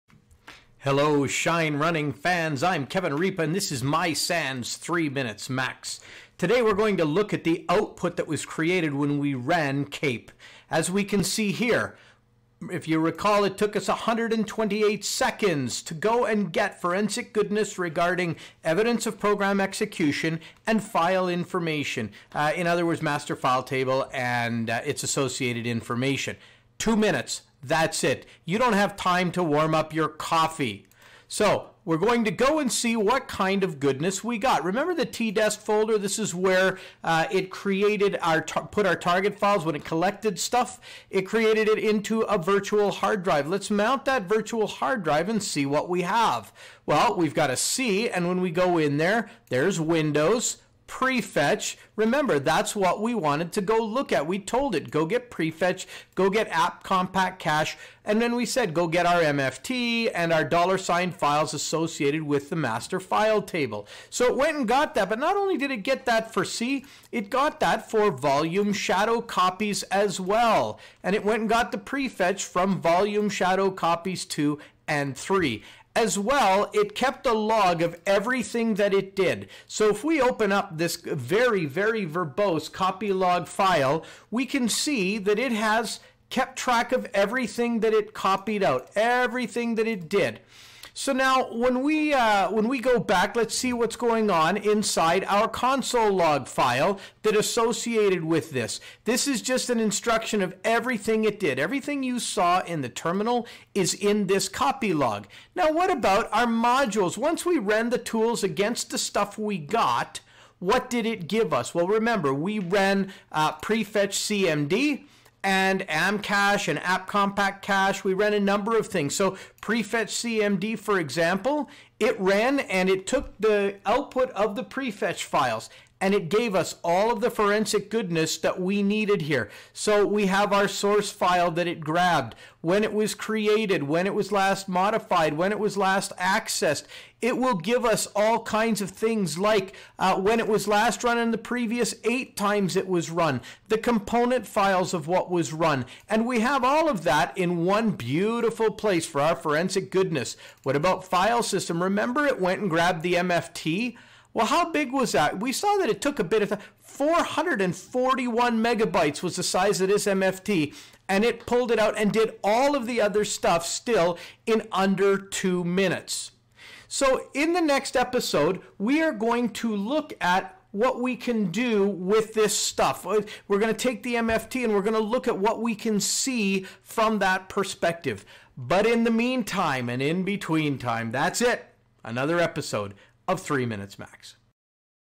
Episode 86: Reviewing the Output Created by KAPE
In this episode, we look at what and how the KAPE target files have extracted the data the examiner has selected, and then we will look at what and how the KAPE module files have parsed the data for use by the forensic examiner.